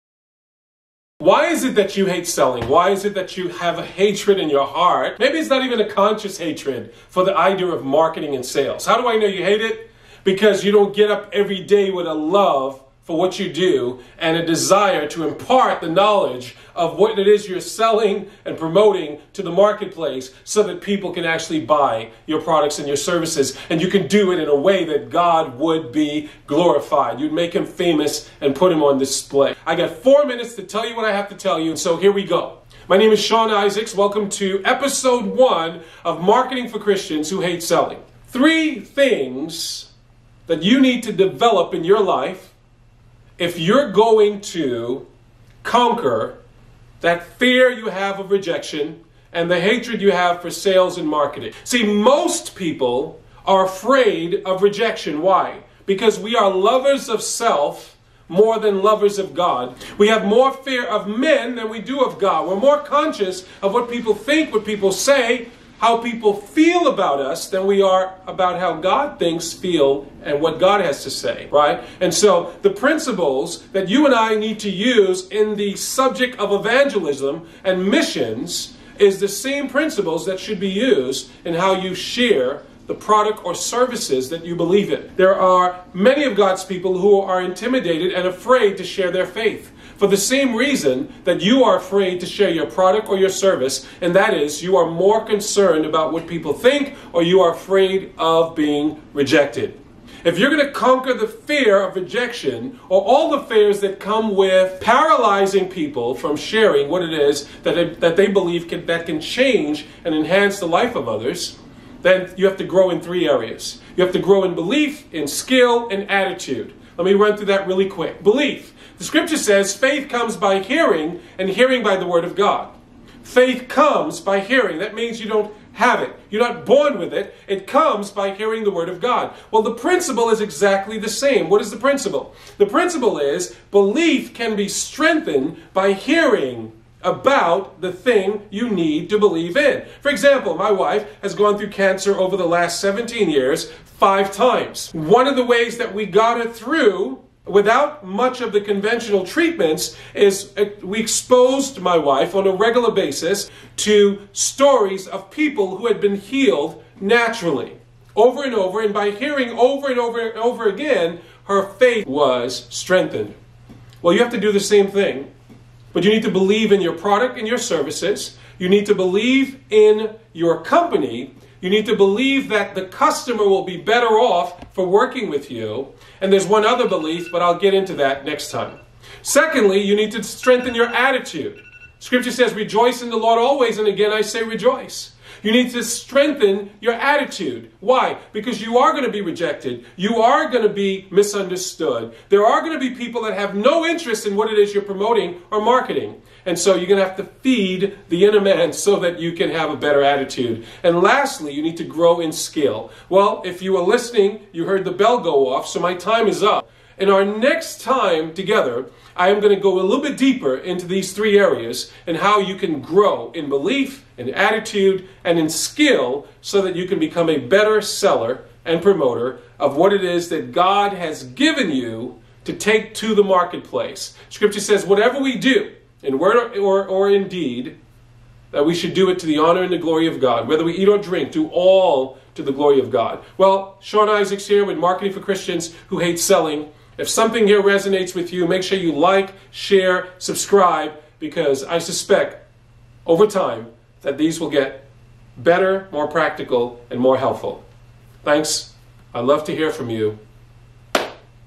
Marketing For Christians | What to do if you hate selling! 3 Areas you must grow in.
If you are not a lover of sales, you need to grow in these 3 areas:

-  BELIEF - What you believe
-  ATTITUDE - How you think
-  SKILLS - How you act